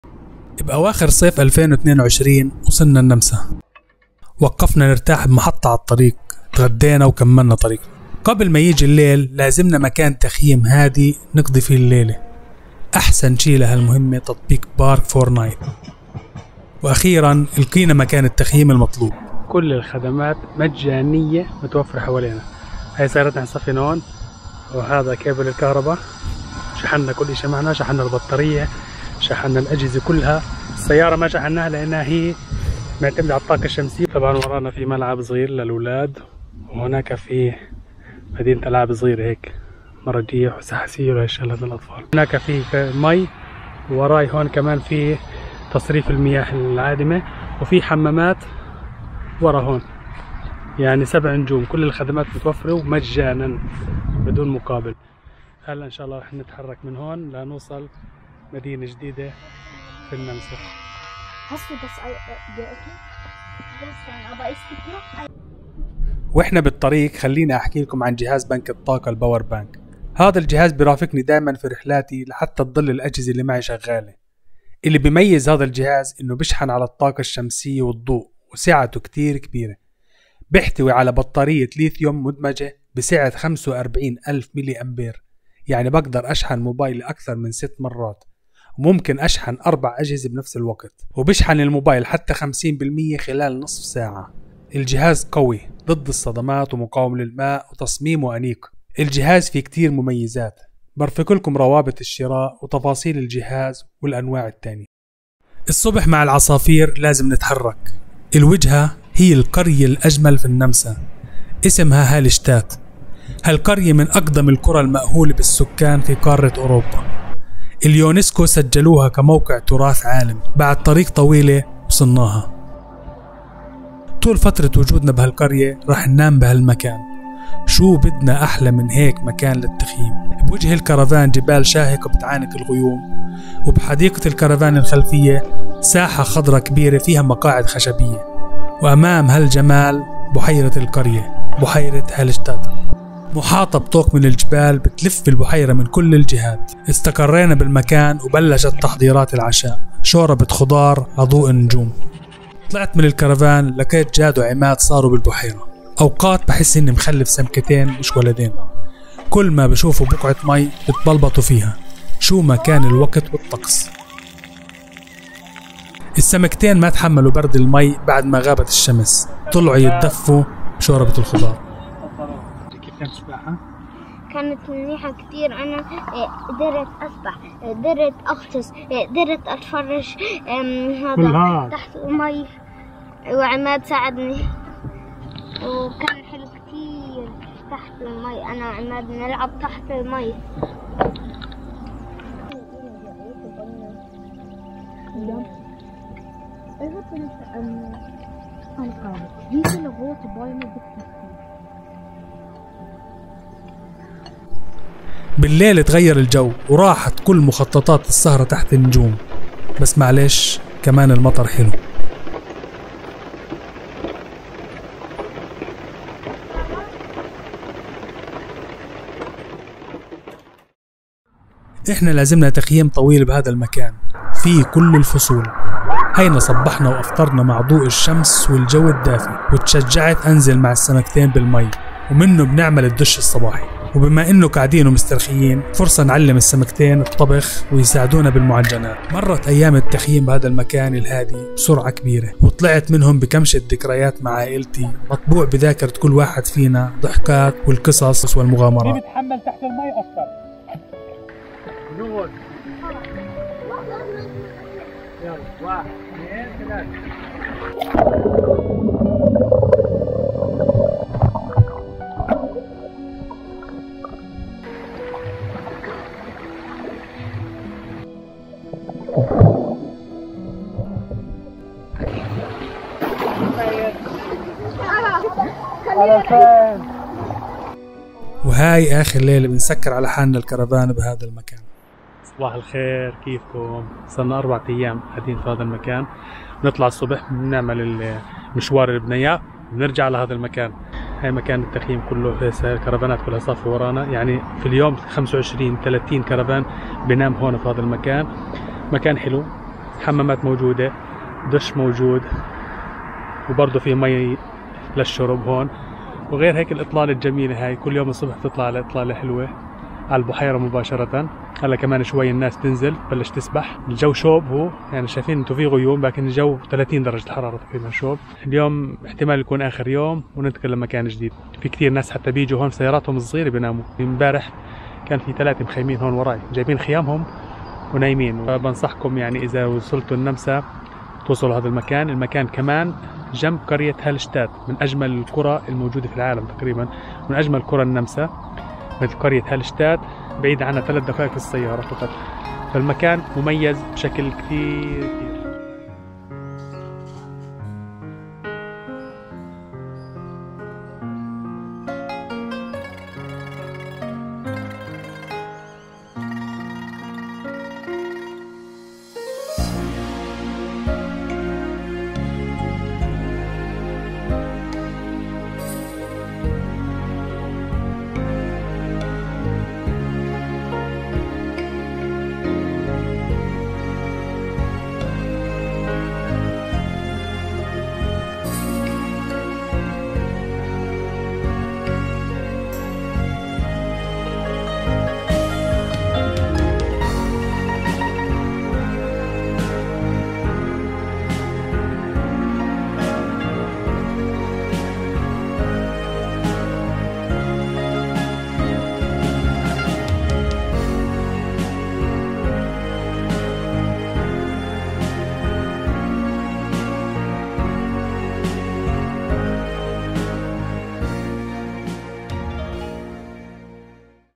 تخييم مجاني في أجمل قرى العالم .. جهاز بنك طاقة Power Bank بنصحكم فيه
الشركة تقدم خدمات البيع بالجملة والتجزئة. لا تتردد في الاتصال،